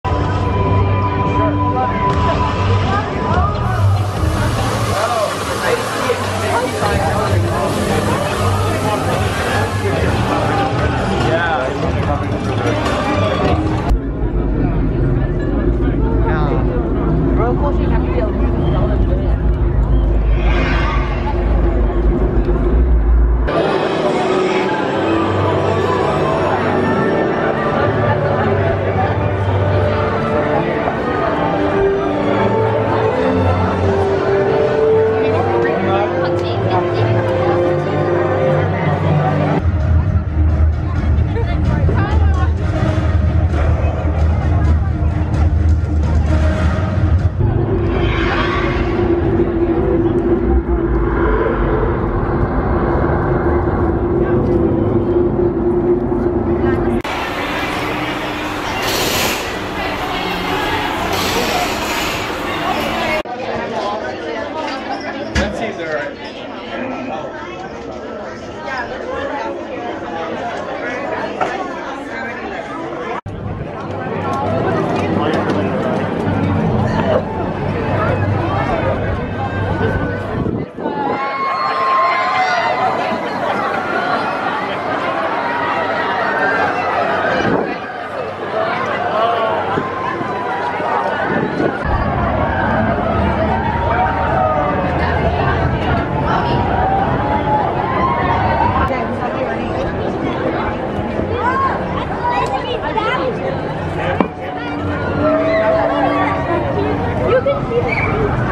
Wizarding World of Harry Potter Off-Ride Footage, Universal Studios Hollywood | Non-Copyright
The Wizarding World of Harry Potter is one of the most immersive themed areas in any theme park.  This can be found at Universal parks across the globe, but this footage is from the version in Hollywood.

0:00- Hogsmeade
0:40- Hogwarts Castle
0:57- Harry Potter and the Forbidden Journey (Kuka Robo Arm Dark Ride)
1:13- Flight of the Hippogriff (Mack Junior Coaster)